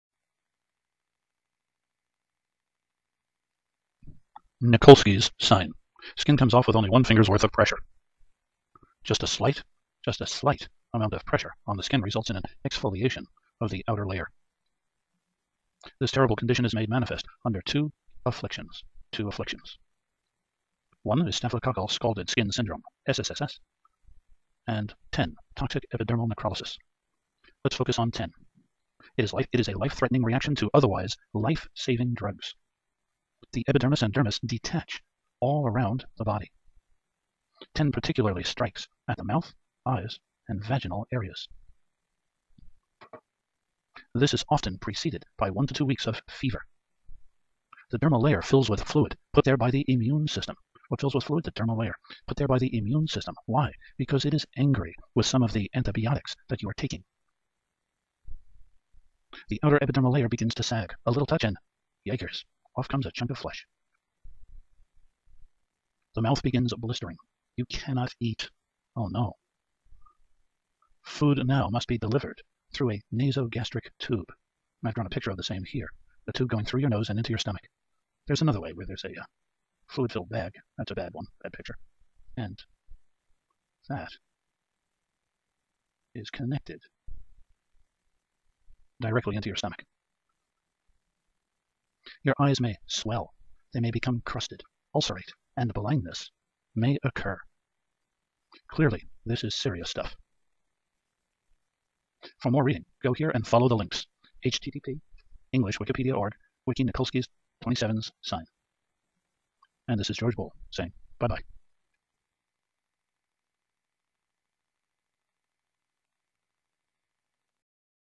Nikolskys Sign
Nikolskys sign is looked at, chiefly in relation to TEN.
(Toxic Epidermal Necrolysis)